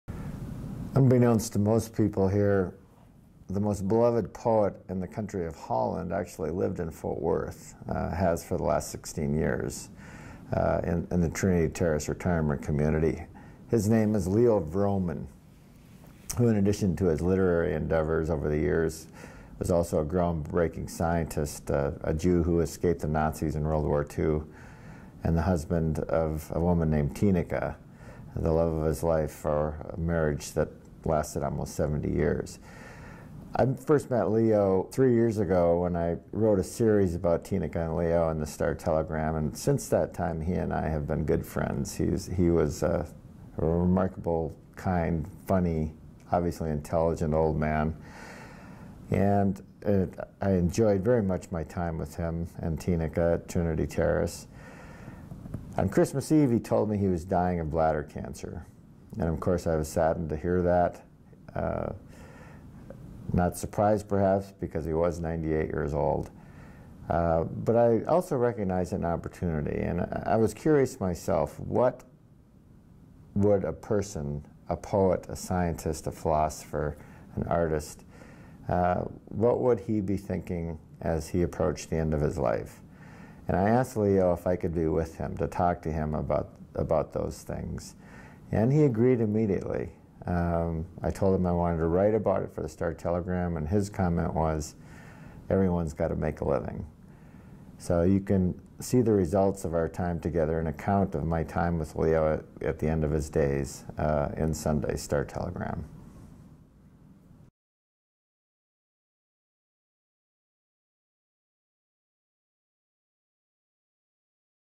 Preview of Tim Madigan's story of Leo Vroman's last days
Tim Madigan previews his loving profile of poet Leo Vroman, who died in early March at 98. Fort Worth Star-Telegram/David Kent